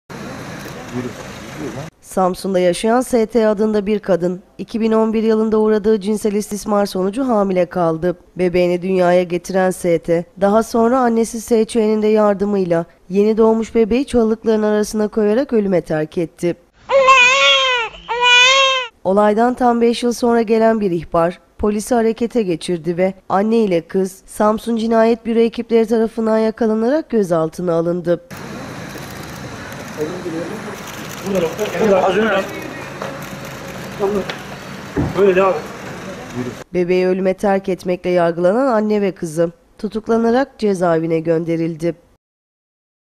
SAMSUN'DA YENİ DOĞMUŞ BEBEĞİNİ ÖLÜME TERK EDEN ANNE TUTUKLANDI...
Samsun'da yaşayan S.T adında bir kadın, 2011 yılında uğradığı cinsel istismar sonucu hamile kaldı.

    Bebeğini Dünya'ya getiren S.T, daha sonra annesi S.Ç'nin de yardımıyla, yeni doğmuş bebeği çalılıkların arasına koyarak ölüme terk etti.

    Olaydan tam 5 yıl sonra gelen ihbar polisi harekete geçirdi ve Anne ile kız Samsun Cinayet Büro ekipleri tarafından yakalanarak gözlatına alındı.